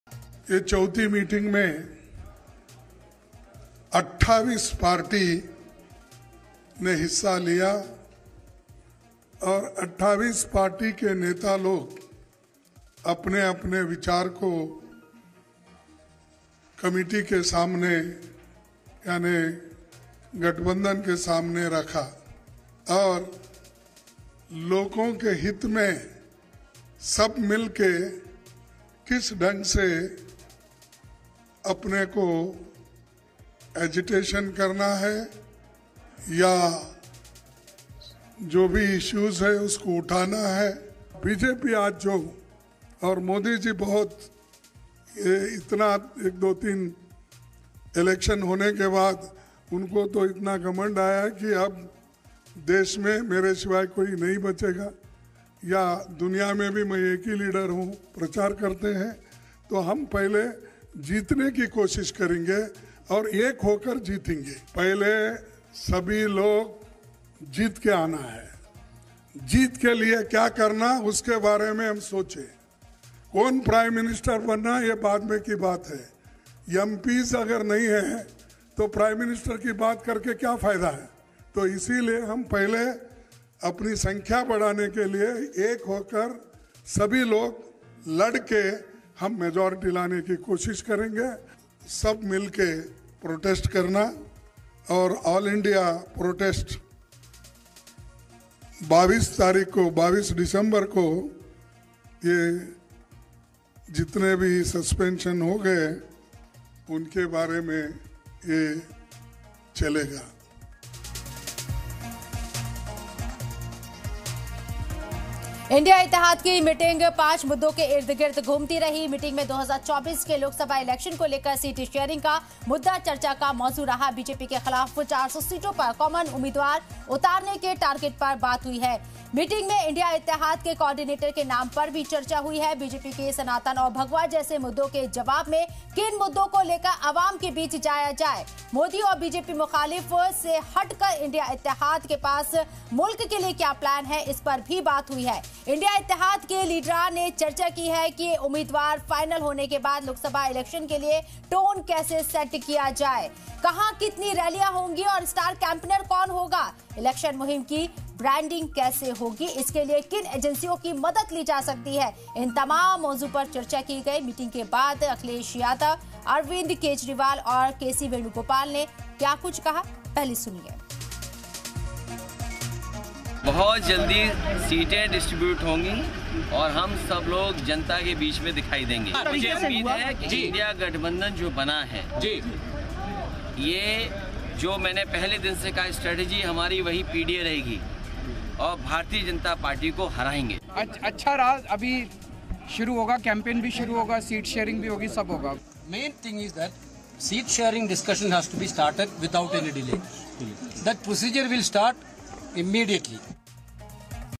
Lok Sabha Election 2024 | सनातन-भगवा मुद्दे के जानिब में अपोज़ीशन के मुद्दे | Alliance Meeting
INDIA Alliance Meeting: पटना, बेंगलुरु और मुंबई के बाद गठबंधन की बैठकों का कारवां अब दिल्ली पहुंच चुका है. दिल्ली के अशोका होटल में  I.N.D.I.A. गठबंधन की बैठक शुरु हुई. लालू यादव, तेजस्वी यादव, नीतीश कुमार, खरगे समेत कई बड़े नेता इस मीटिंग में उपस्थित हैं. इस बैठक में सीटों की शेयरिंग समेत प्रधानमंत्री उम्मीदवार के नाम पर भी चर्चा होने की संभावना है. 26 दल के प्रमुख इस बैठक में शामिल हुए

After Patna, Bengaluru and Mumbai, the caravan of alliance meetings has now reached Delhi. I.N.D.I.A. at Ashoka Hotel, Delhi. The alliance meeting started. Many big leaders including Lalu Yadav, Tejashwi Yadav, Nitish Kumar, Kharge are present in this meeting. There is a possibility of discussion on the name of the Prime Ministerial candidate along with sharing of seats in this meeting. 26 party heads attended this meeting